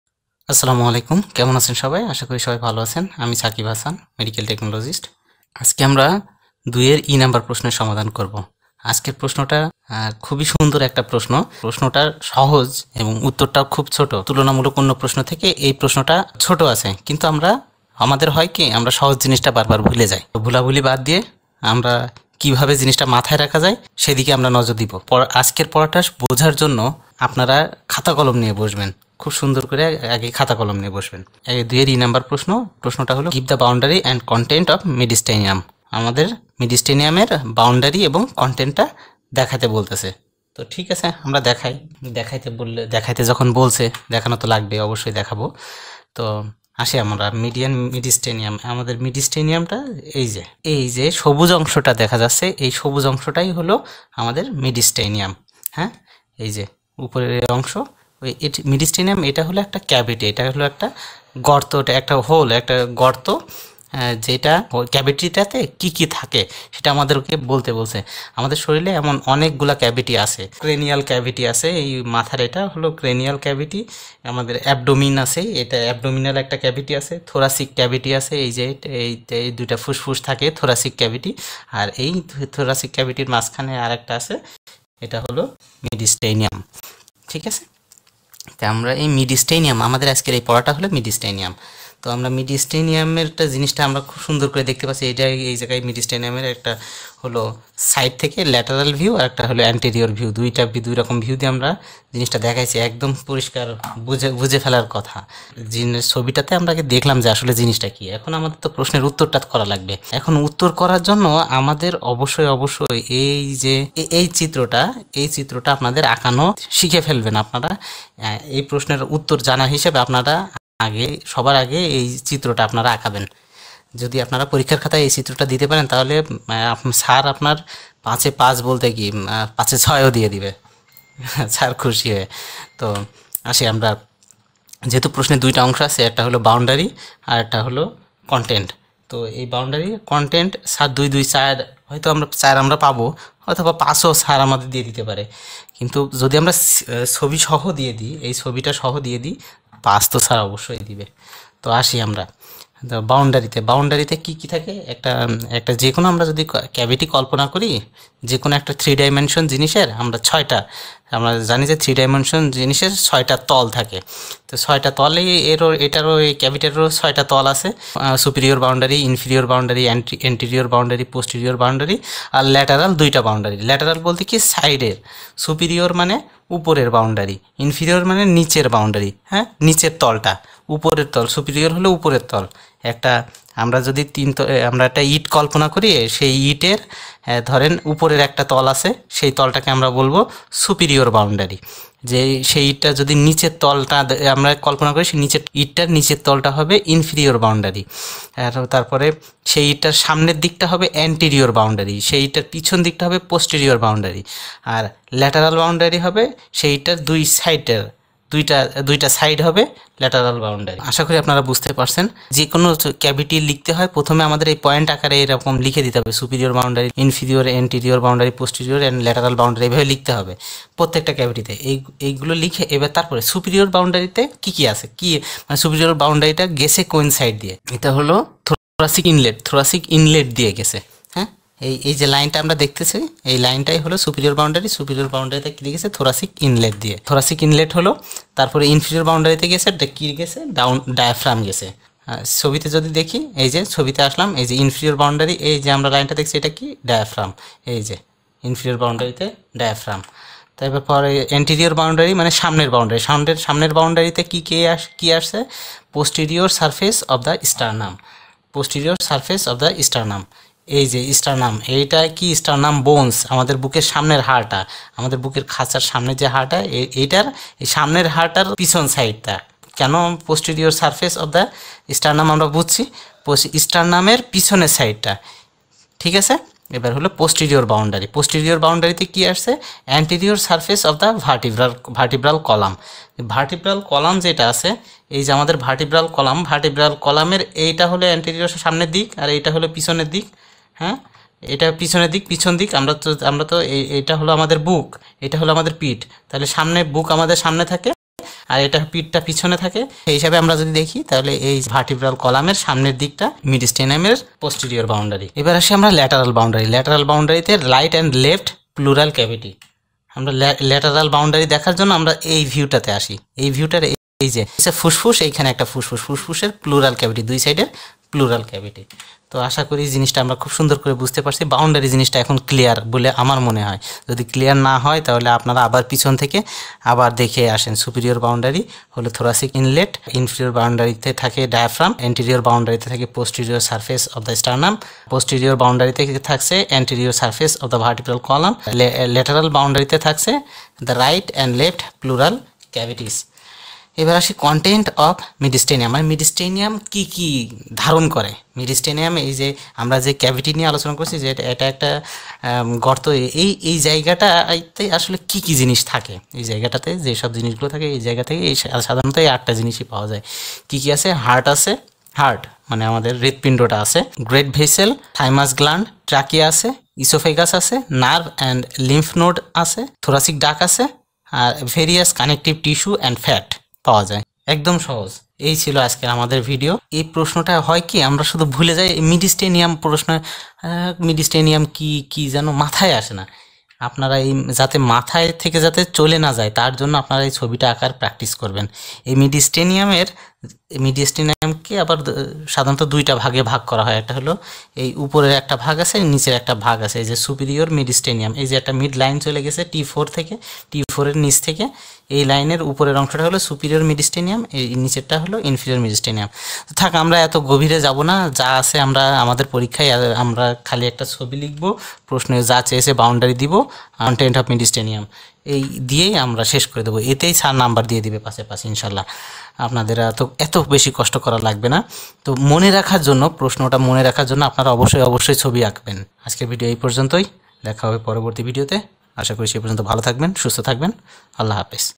Give the boundary and content of mediastinum. || Jan 2020 Anatomy 2(e)
IHT Question Solve Jan 2020  Anatomy 2(e) || Give the boundary and content of mediastinum.
Hello Everyone
I am Sakib Hasan (Medical technologist)
Search on Youtube and Facebook :- sakibhasan08
Thanks